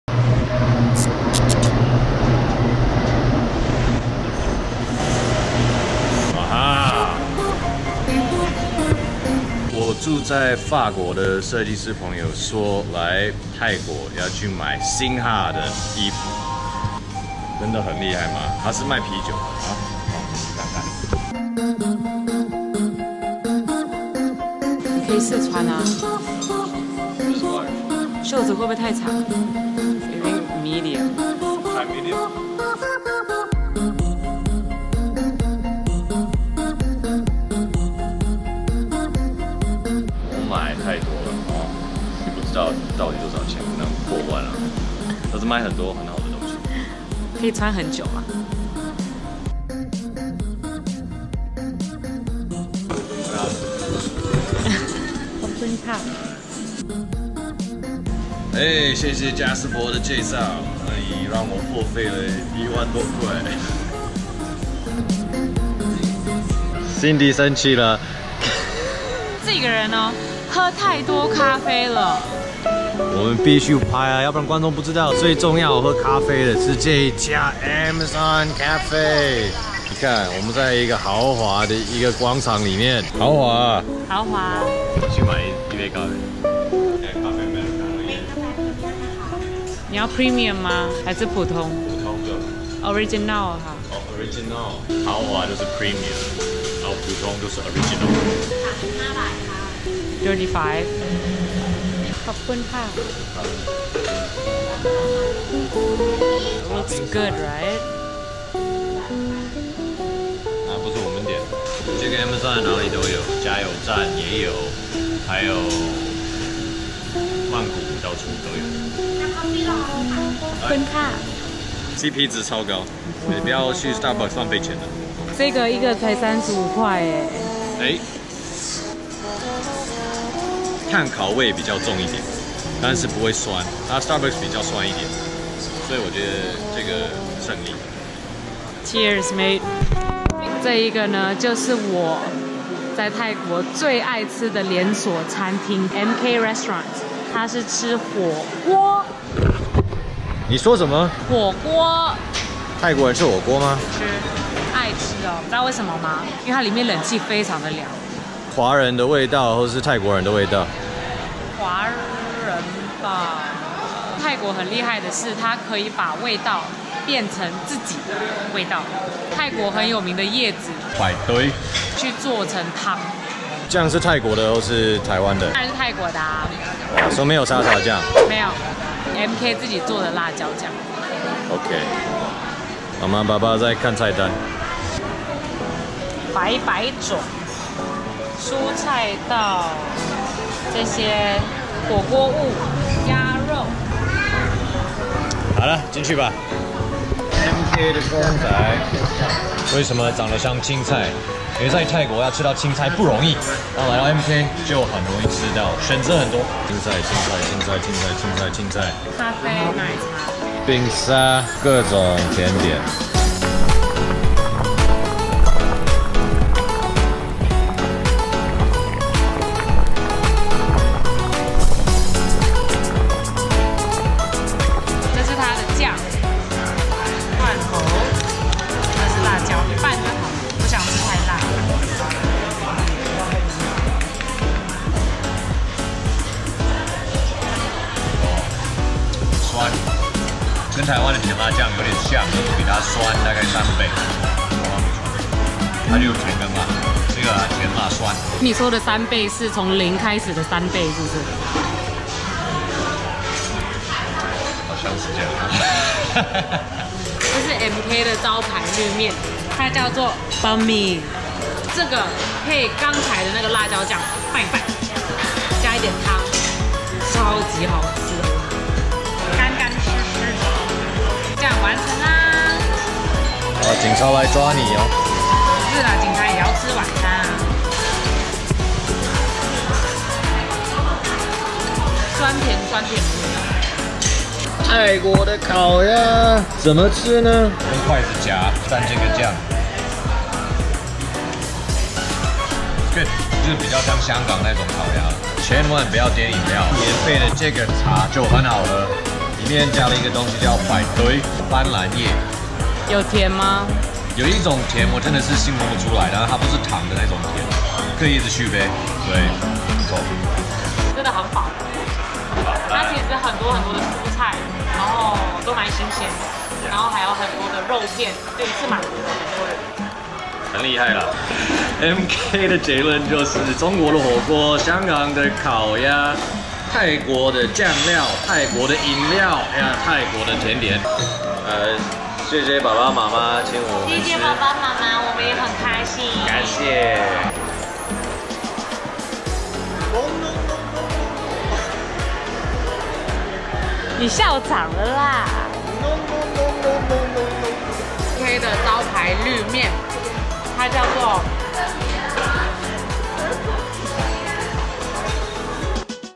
VLOG11 曼谷N訪3家連鎖店 3 Thai Franchises that will save you money.
班傑在泰國這幾間餐廳真的N訪...不誇張。在泰國除了玩水，最重要的行程一定還要加上逛街和吃!!! 曼谷3家知名、CP又值爆表的連鎖店（我們去的地方是曼谷市中心Siam），讓你血拼、吃飽、喝足~

Benji and Cindy take you to shop eat and drink at thailands local franchises.  Get the best bang for your buck by shopping eating and buying coffee the thai way.